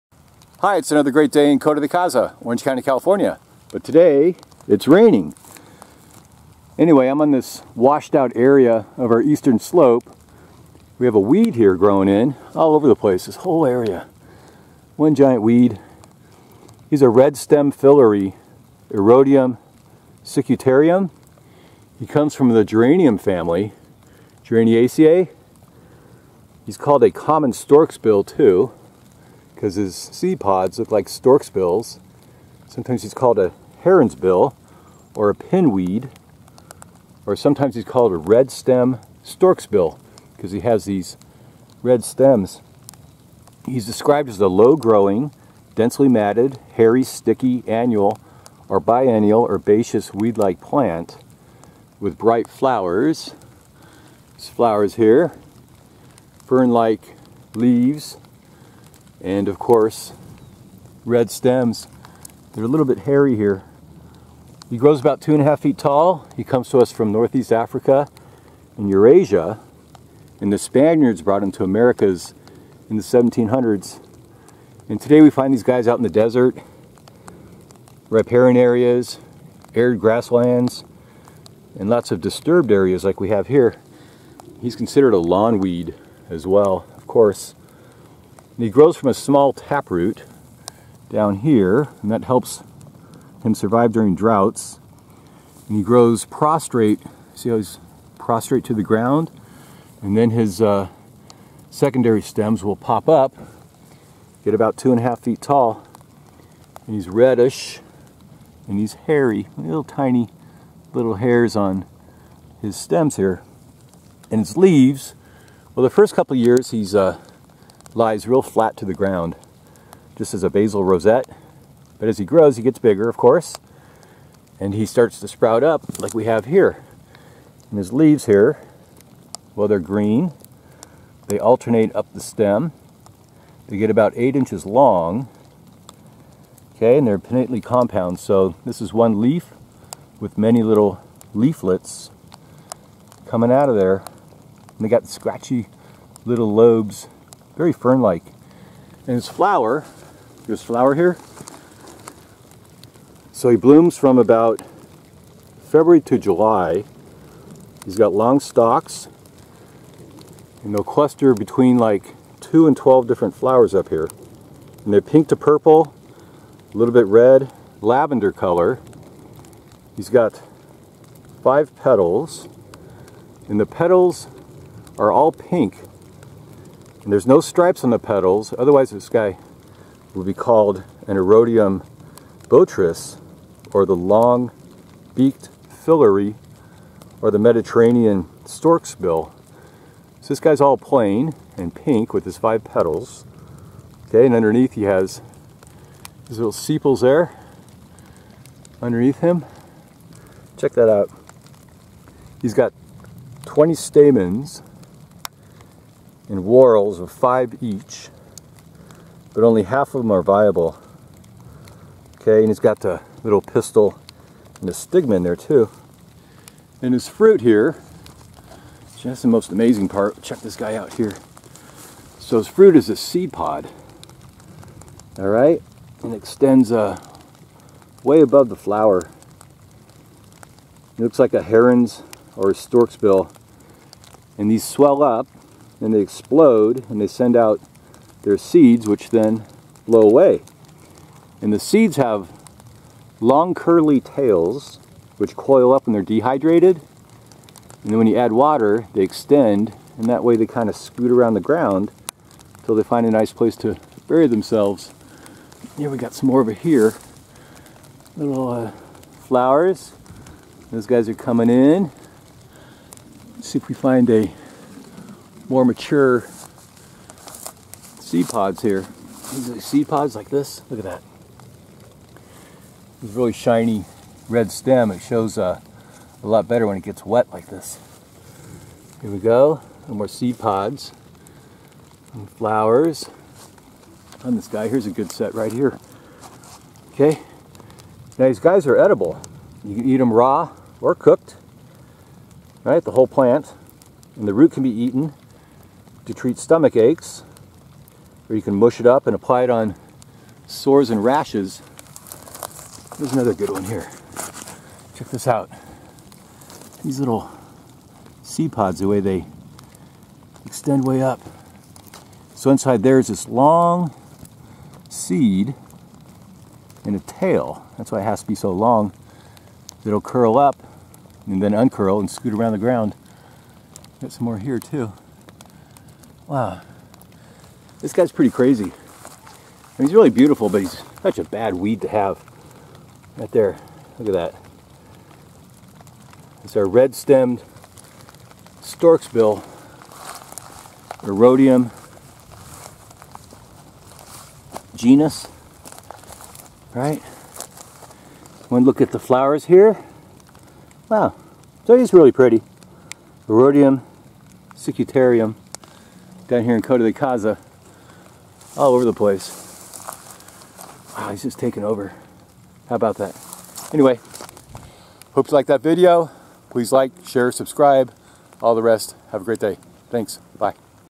REDSTEM FILAREE California wildflowers, Erodium cicutarium; Common Stork's Bill,Heron's Bill,Pinweed
Narrative on our REDSTEM FILAREE California Wildflowers, Erodium cicutarium; also known as Common Stork's Bill, Heron's Bill, Pinweed, Redstem Stork's Bill. Common weed in Southern California, Coto de Caza.  Do not confuse with the Erodium botrys or Long-beaked filaree or Mediterranean stork's bill.